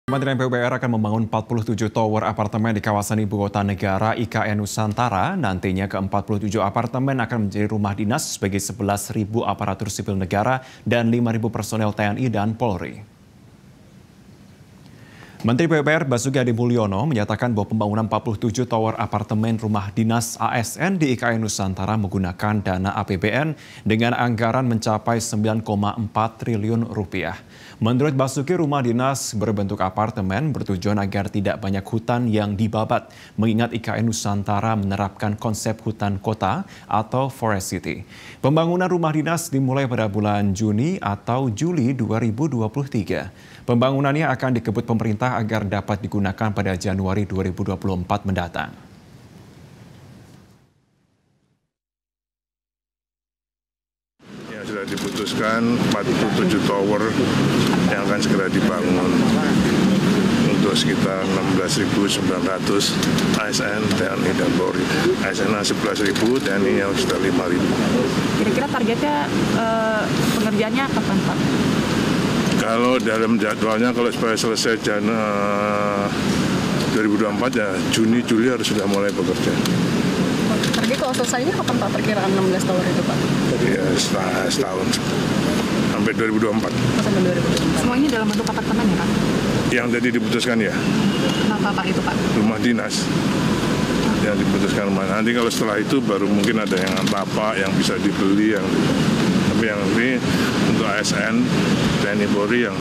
PUPR akan Bangun 47 Tower Apartemen di IKN untuk ASN
MetroTV, Kementerian PUPR akan membangun 47 tower apartemen di Kawasan Ibu Kota Negara (IKN) Nusantara. Nantinya ke-47 apartemen akan menjadi rumah dinas bagi 11 ribu aparatur sipil negara dan 5 ribu personel TNI dan Polri.

Menteri PUPR Basuki Hadi Muljono mengatakan, pembangunan 47 tower apartemen rumah dinas ASN di IKN Nusantara menggunakan dana APBN dengan anggaran mencapai Rp9,4 triliun. Menurut Basuki, rumah dinas berbentuk apartemen bertujuan agar tidak banyak hutan yang dibabat, mengingat IKN Nusantara menerapkan konsep hutan kota atau forest city.

Pembangunan rumah dinas dimulai pada Juni atau Juli 2023. Pembangunannya akan dikebut pemerintah agar dapat digunakan pada Januari 2024.